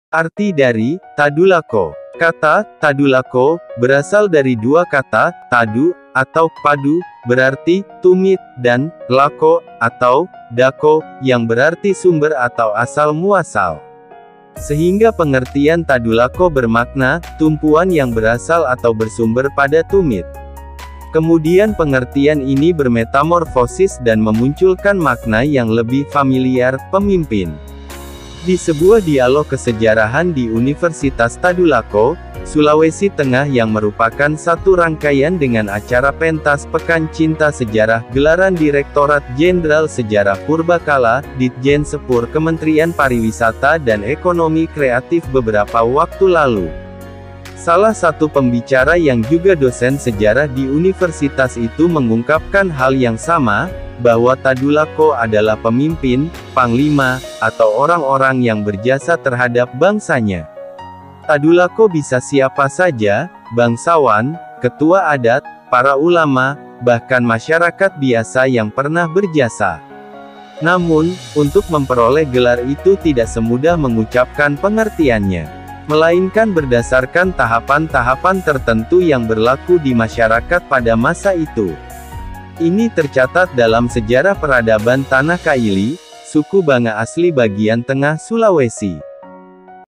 Sejarah Arti Kata Tadulako
mengenang sejarah Tadulako di Sulawesi tengah

semoga degan Vidio ini bisa menambah ilmu pengetahuan tentang arti & Makna TADULAKO ' SALAM BUDAYA BUMI TADULAKO SULAWESI TENGAH PALU